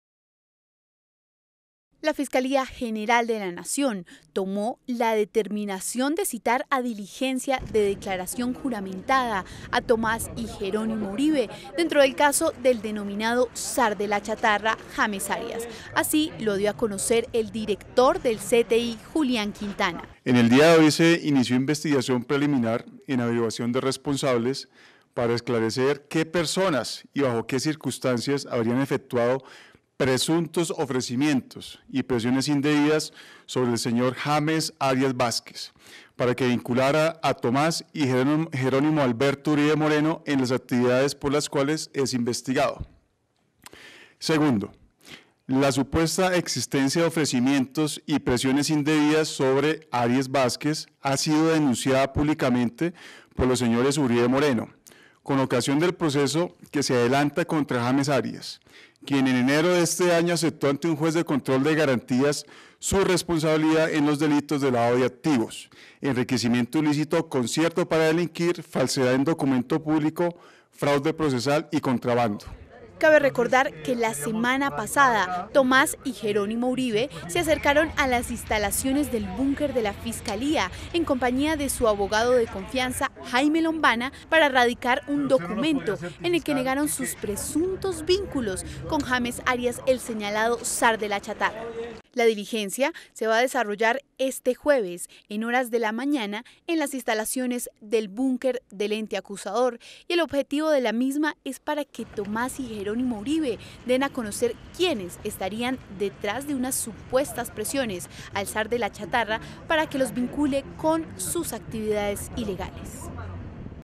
Tomás y Jerónimo Uribe rinden mañana versión, bajo juramento, ante la Fiscalía Noticias RPTV
La Fiscalía General de la Nación citó a declarar, bajo la gravedad de juramento, a los hijos del ex Presidente y senador por el Partido Centro Democrático Álvaro Uribe Vélez, Tomás y Jerónimo Alberto Uribe Moreno, el próximo jueves 10 de marzo, como parte de la investigación preliminar abierta por la denuncia que hicieron ellos para que se esclarezca quiénes son los responsables y bajo qué circunstancias se presentaron los posibles ofrecimientos y presiones indebidas para inculparlos, hechas por el detenido Jaime Arias Vásquez, conocido como “El Zar de la Chatarra”.
RPTV